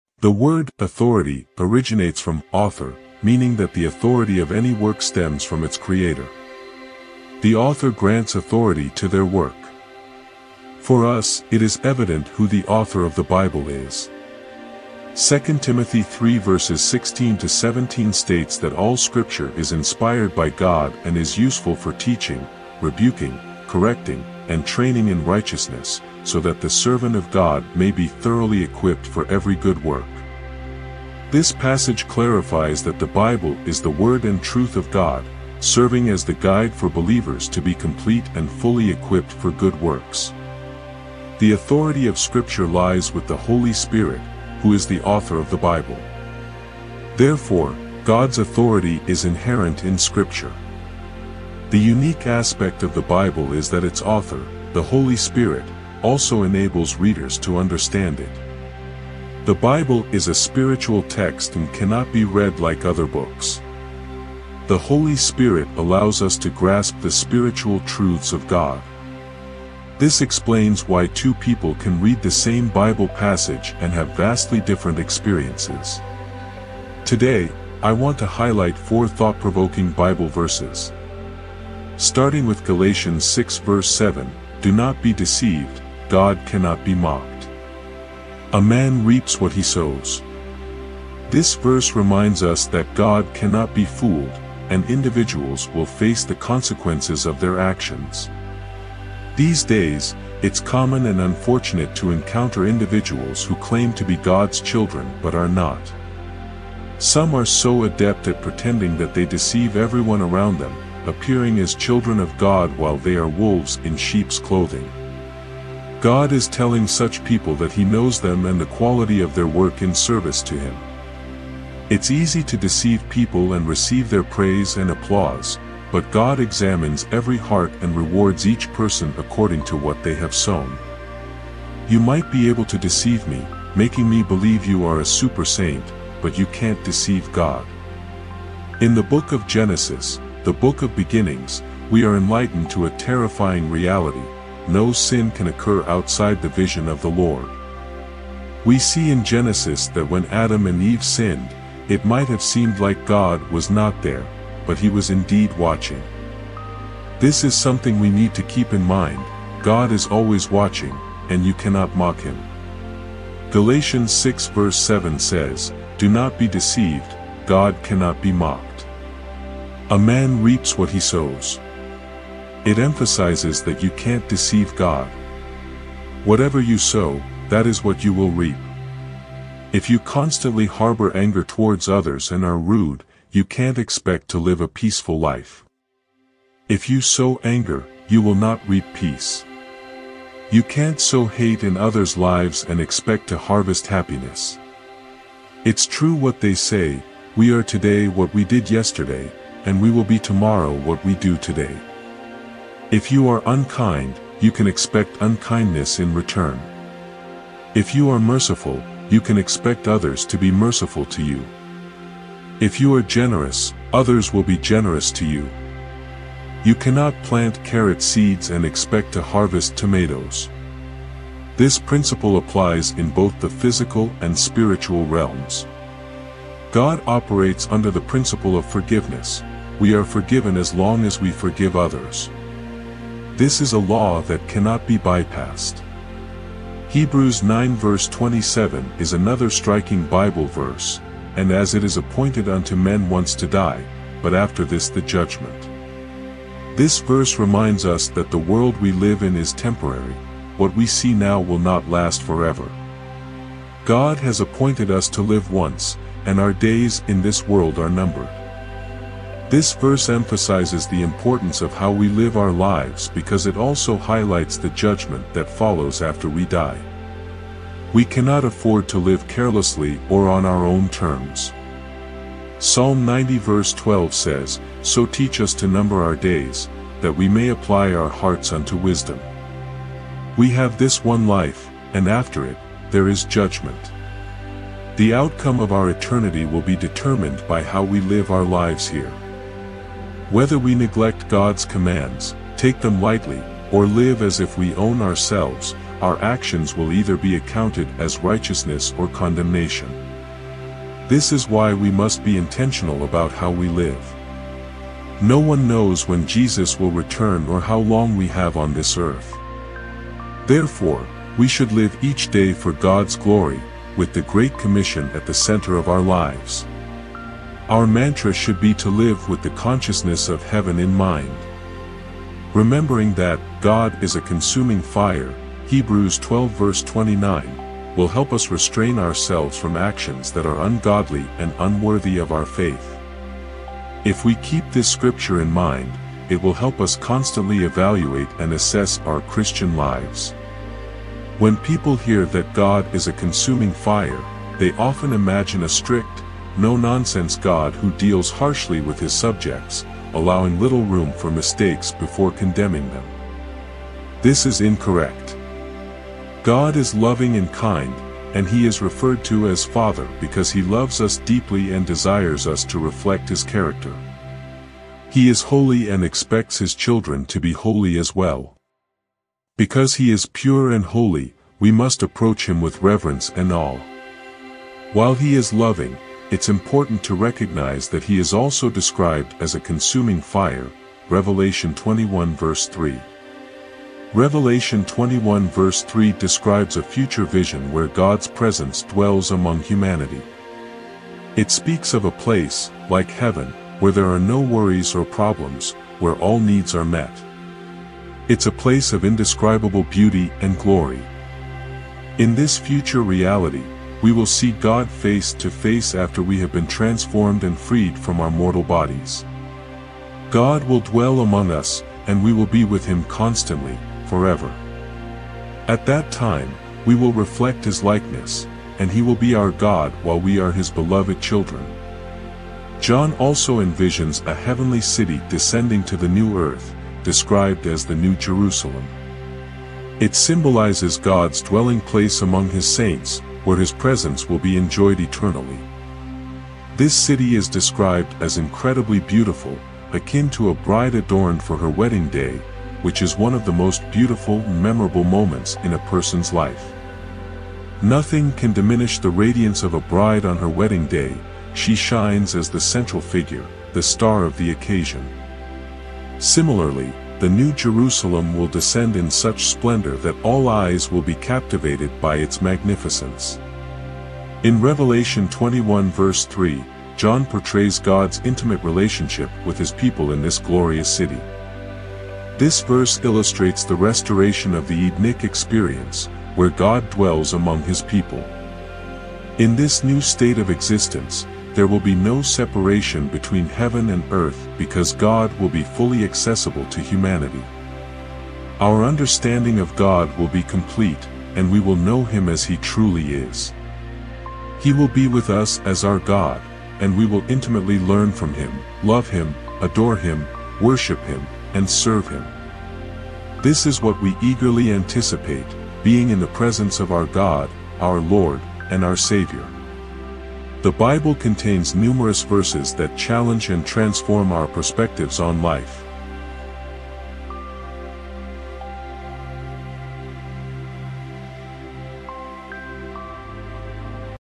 4 BIBLE VERSES that CHANGED My Whole LIFE | 4 POWERFUL VERSES
In this impactful video, I share 4 Bible verses that have profoundly transformed my life. These verses hold immense power and wisdom, offering guidance, comfort, and inspiration in times of need. Join me as we explore their meaning and significance, and discover how they can bring positivity and transformation to your life too.